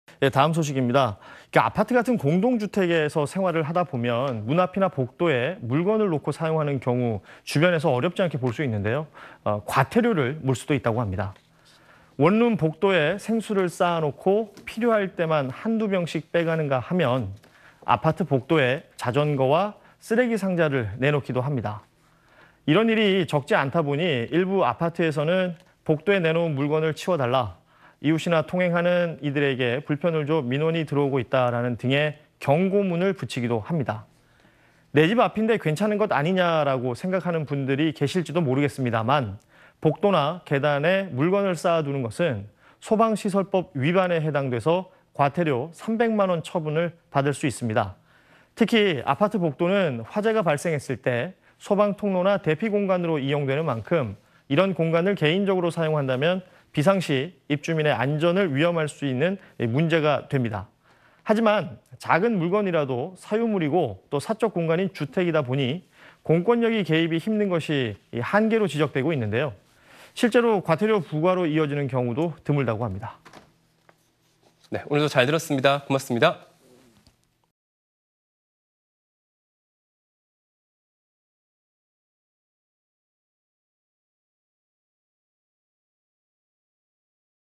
"내 집 앞인데 뭐~" 복도에 물건 놓으면 과태료 대상 / SBS / 뉴스딱
아파트 같은 공동주택에서 생활을 하다보면 문 앞이나 복도에 물건을 놓고 사용하는 경우, 주변에서 어렵지 않게 볼 수 있는데요.
과태료를 물 수도 있다고 합니다.
원룸 복도에 생수를 쌓아놓고 필요할 때만 한, 두 병씩 빼가는가 하면 아파트 복도에 자전거와 쓰레기 상자를 내놓기도 합니다.